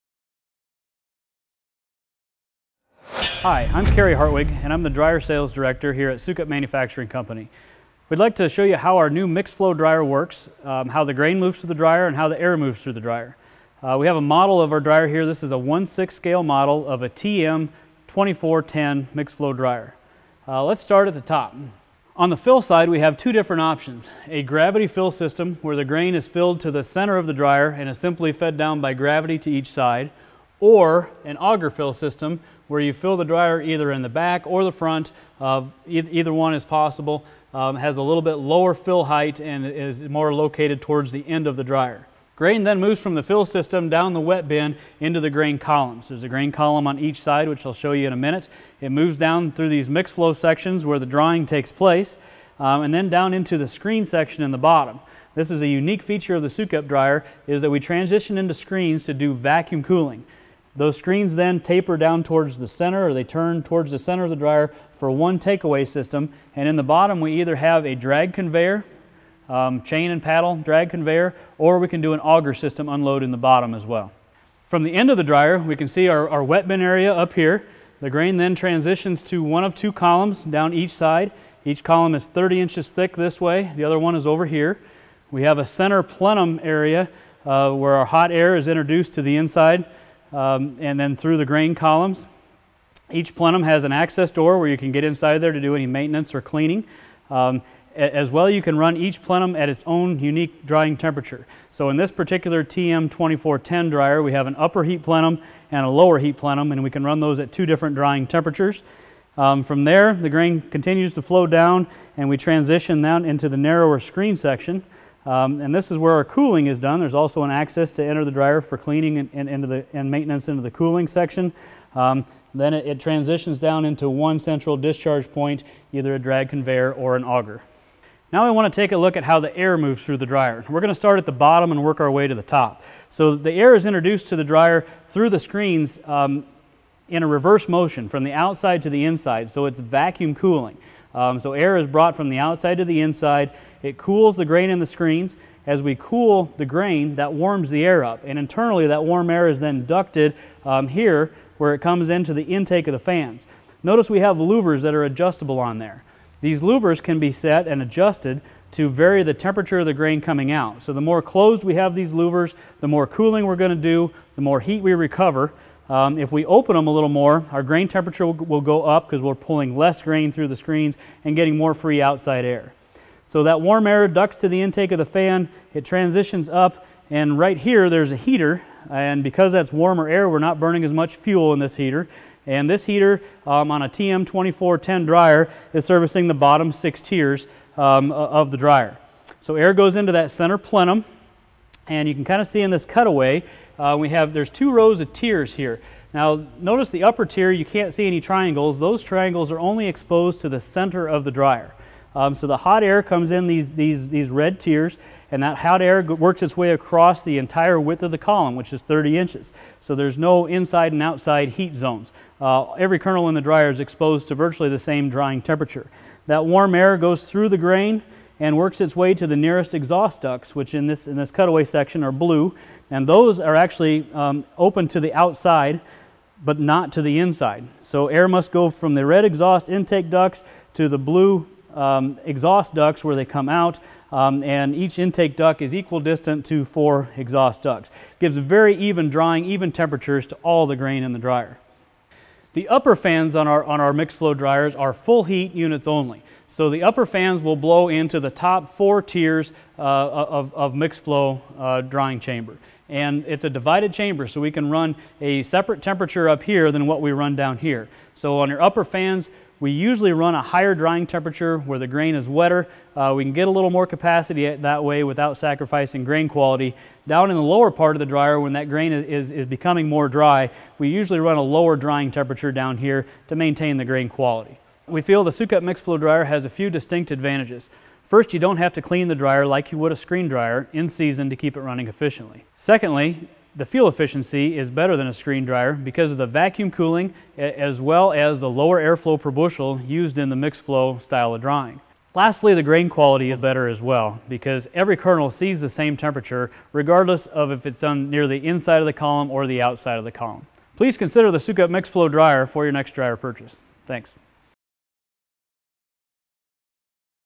Mixed Flow dryer tutorial V2
Kerry Hartwig uses a 1/6th scale model of a Sukup Mixed Flow Dryer to explain how it works.